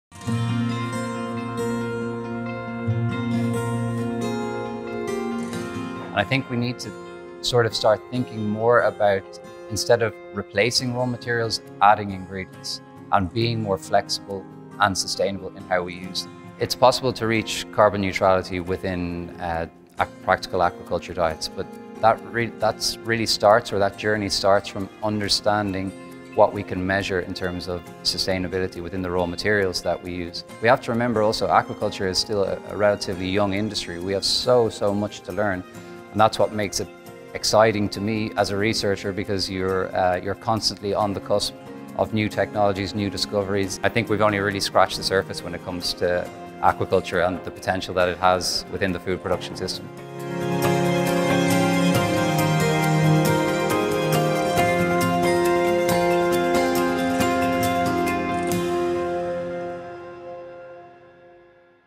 Philip Lyons on Sustainability in Aquaculture
Philip Lyons, global manager of aquaculture research at Alltech Coppens, discusses sustainability in aquaculture